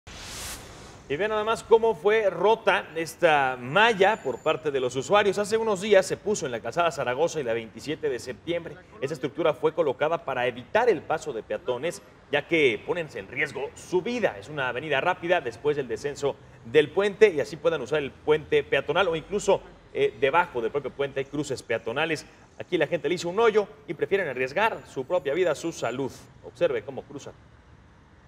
Rompen malla en Calzada Zaragoza
Peatones rompieron la malla ciclónica colocada en la Calzada Zaragoza para evitar cruzar por el puente.